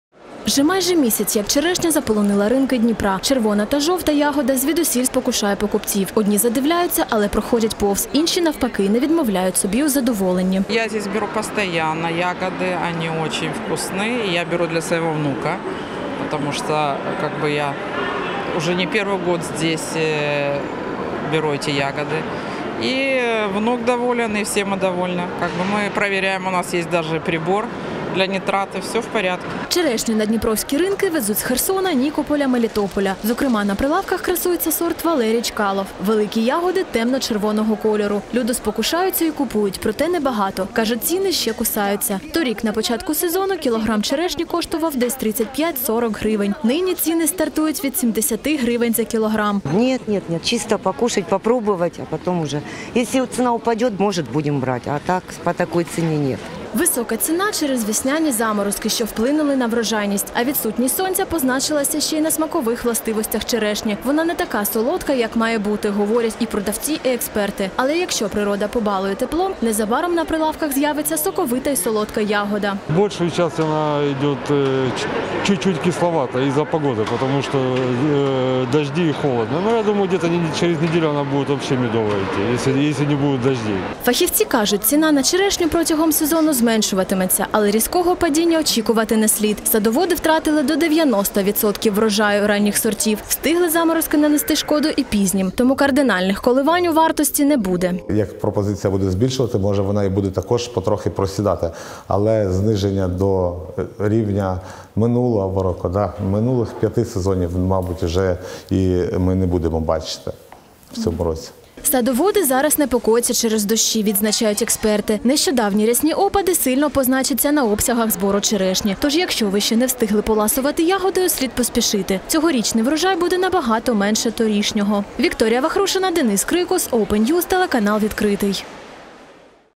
Ягоди менше та й ціни кусаються: в Україні розпочався черешневий сезон
Ціни кусаються. Попри весняні заморозки та несприятливу погоду, черешня на дніпровських ринках з’явилася вчасно. Однак вартість ягоди покупців збентежила. Вона вдвічі вища за минулорічну. Десь 70 грн. за кілограм. Причина -  холоди та мало сонця для визрівання, кажуть експерти. Це вже вилинуло на врожайність ранніх сортів черешні. Чи позначиться це на обсягах збору пізніх сортів і чи варто очікувати значного здешевлення, з’ясовували журналісти Відкритого.